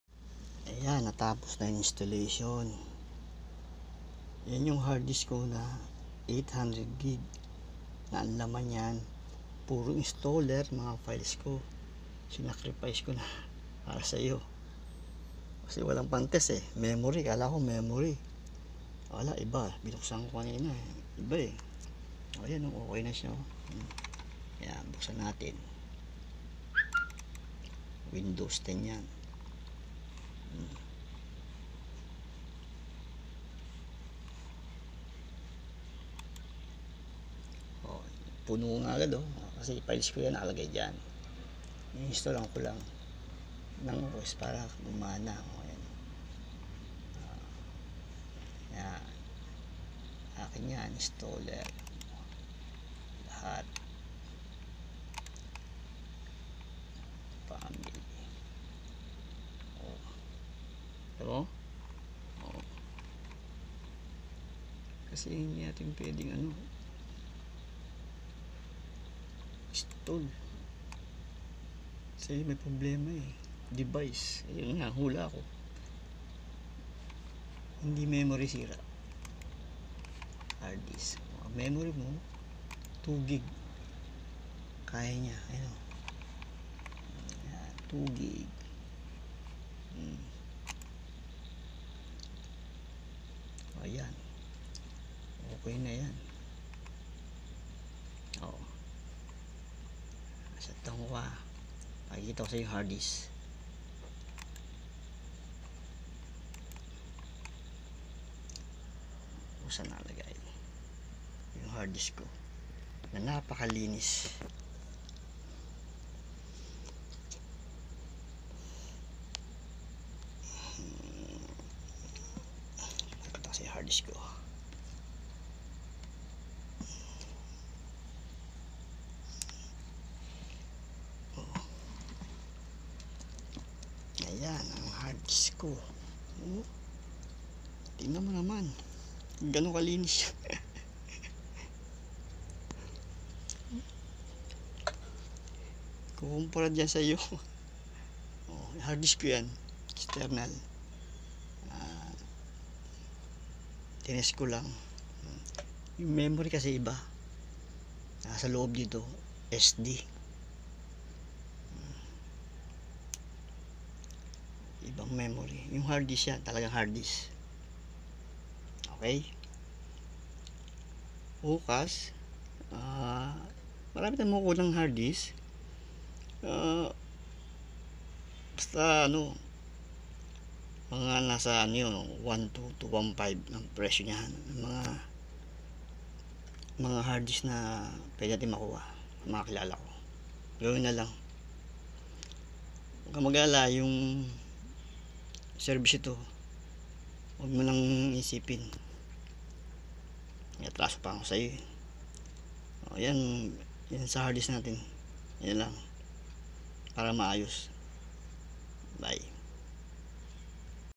Acer Aspire T77h462 "LCD Problem and Hardisk" Part4
Acer Aspire T77h462
LCD Problem-Auto off and lines
Hardisd-Window 7,8 &10 not continue to install...